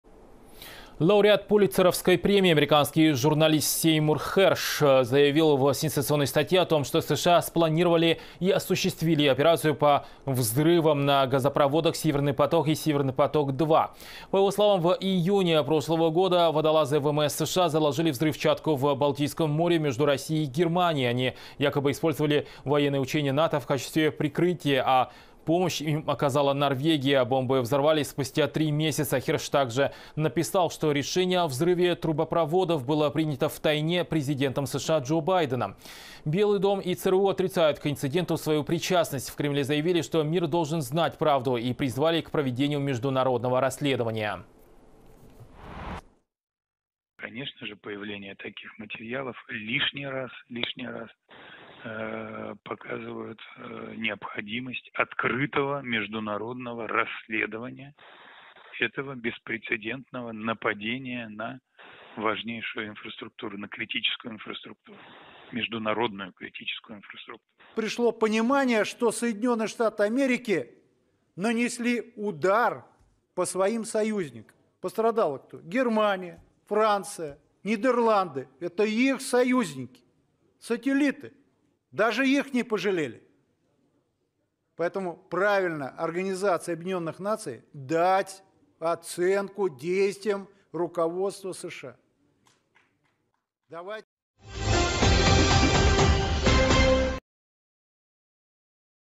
"Северные потоки" подорвали США? Сенсационное расследование журналиста
Лауреат Пулитцеровской премии, американский журналист Сеймур Херш написал статью, в которой заявил, что диверсию на "Северных потоках" спланировали и осуществили США. По его словам, в июне прошлого года водолазы ВМС Соединенных Штатов заложили взрывчатку в Балтийском море между Россией и Германией. Они якобы использовали военные учения НАТО в качестве прикрытия, а помощь оказала Норвегия. Взорвали эти бомбы спустя три месяца, а приказ, по данным Херша, отдал сам Джо Байден. Белый дом и ЦРУ свою причастность отрицают, в Кремле же призывают к международному расследованию.